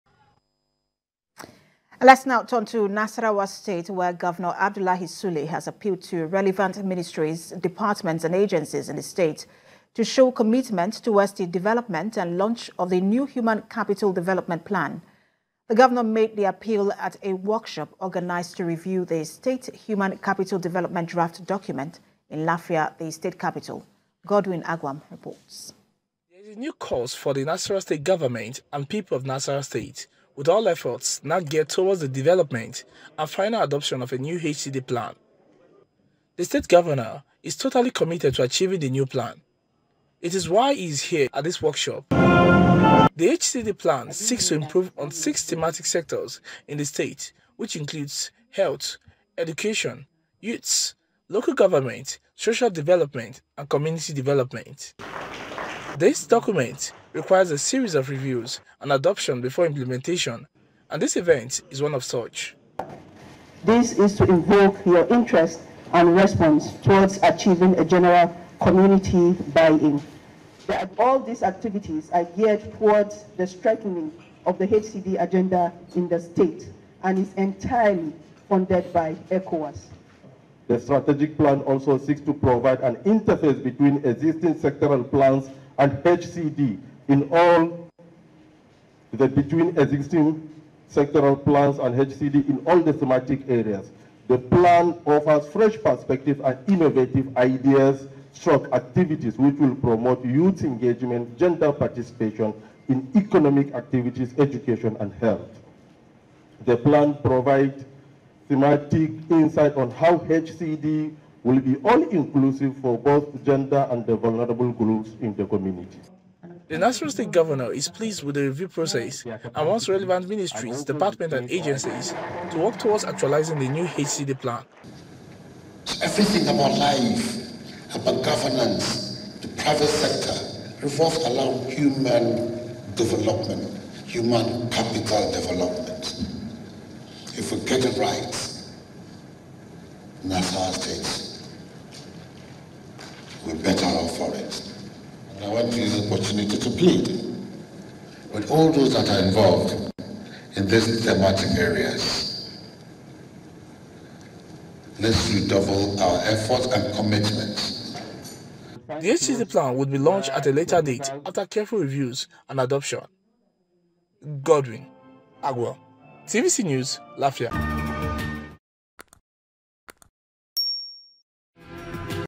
Gov Sule Tasks MDAs To Support New Human Capital Development Plan
Nasarawa State Governor Abdullahi Sule has appealed to relevant Ministries, Departments and Agencies in the state to show commitment towards the development and launch of the new Human Capital Development Plan.
The governor made appeal at a workshop organized to review the state Human Capital Development draft document in Lafia, the capital of the state.